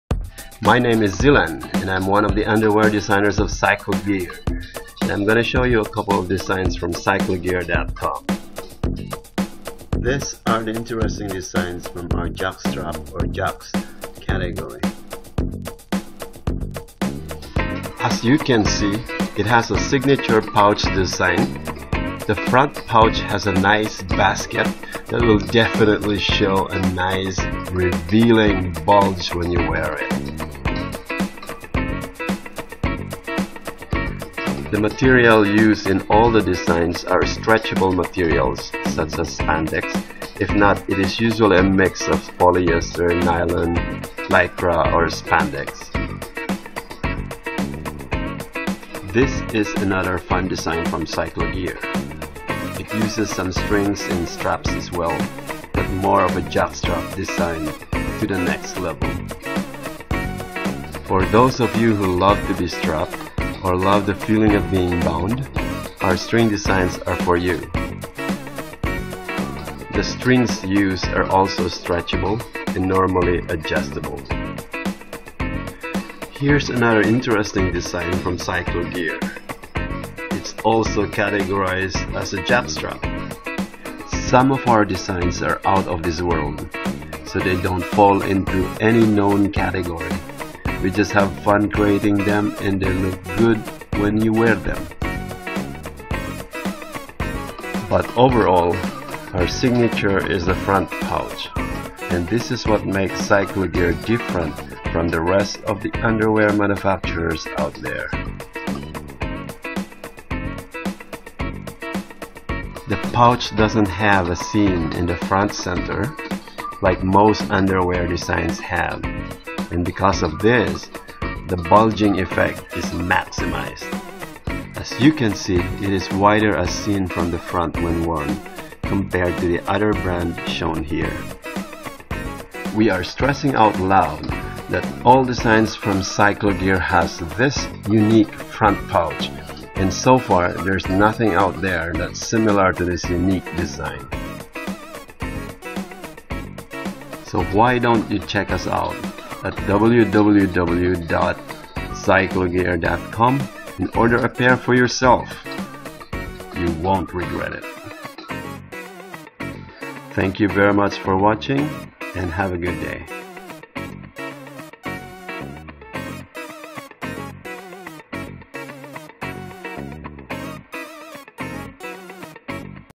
Part 4: Unique Pouch Design Jocks Underwear from ZycloGear
Unique Pouch Design Jocks Underwear for men from ZycloGear that are very sexy and uniquely designed. The pouch is designed with no seam in the front center which makes a nice bulge when you wear them.